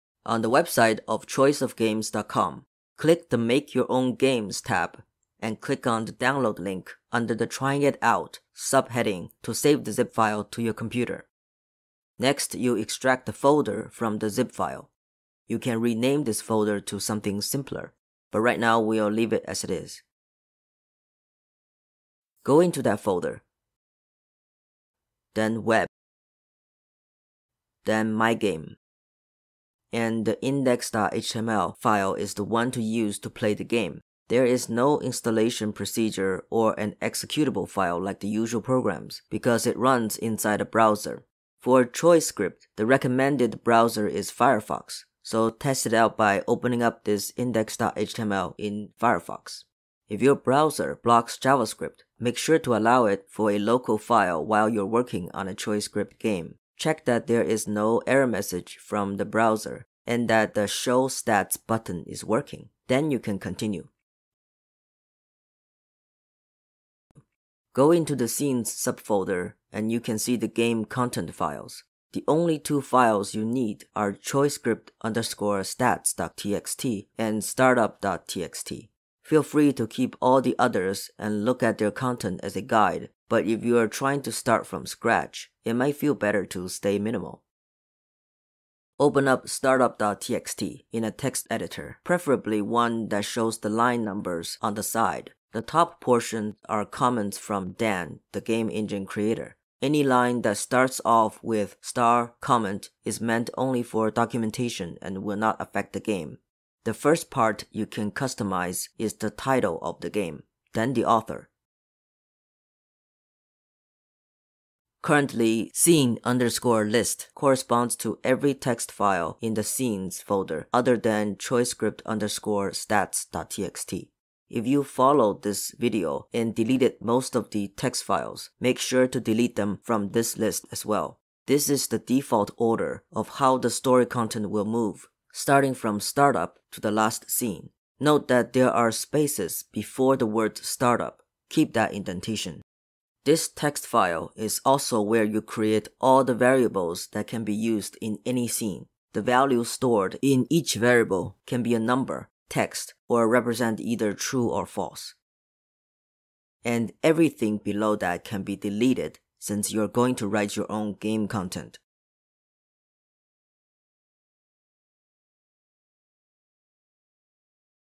(OUTDATED, see description for detail) [ChoiceScript] Basic Setup
OUTDATED due to browser stricter security rules: According to forum post
newer versions of Google Chrome and Firefox have adopted stricter rules regarding javascript (the underlying programming language for ChoiceScript), and thus this tutorial is completely outdated now. I'll need to keep an eye on progress to how the developer will update their code, or look for alternatives to work with ChoiceScript.

[July 14, 2019] A fan-made web-based ChoiceScript coding environment still works for more info). The web version requires a DropBox account and you granting permission for it to write to your DropBox account. At the time of writing, it works in Chrome version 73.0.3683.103 and Firefox version 68. If you use Windows, there is also a 64bit executable file you can install on your machine to work without needing a DropBox account I can't test the Mac version right now. I need the web option because my main work machine uses Linux.
original video description
Note:
In the default scenes folder, ending.txt has the final line "*ending" that terminates the game/story progression, so if player doesn't make the choice that leads to the bad outcome, it won't continue moving the story toward the death.txt scene.